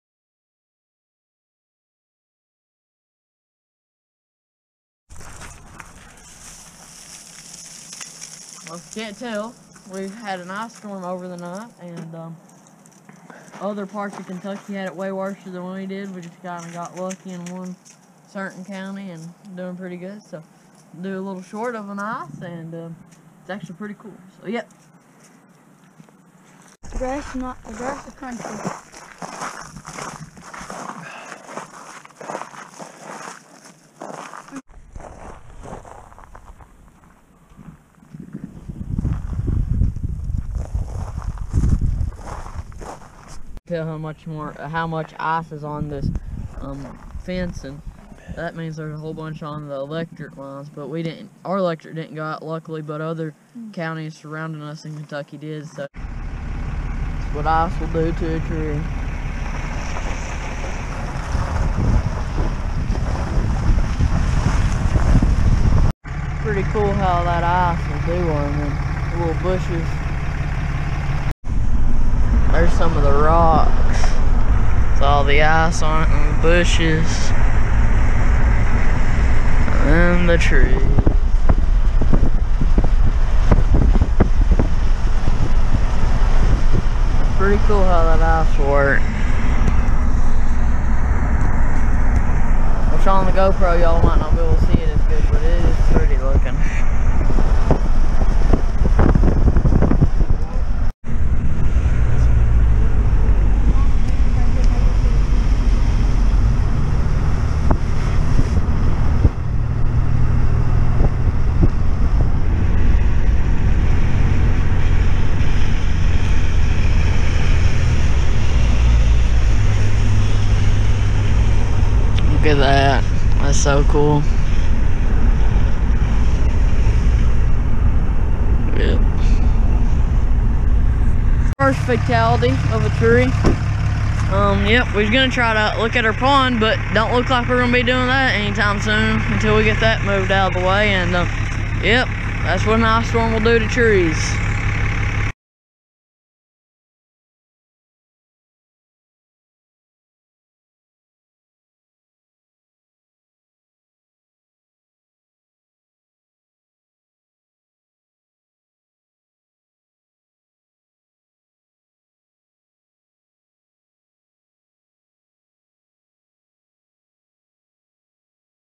Kentucky ICE STORM 2021!
We got a lot of ice but not as bad as surrounding counties. A lot of Kentucky counties have power outages!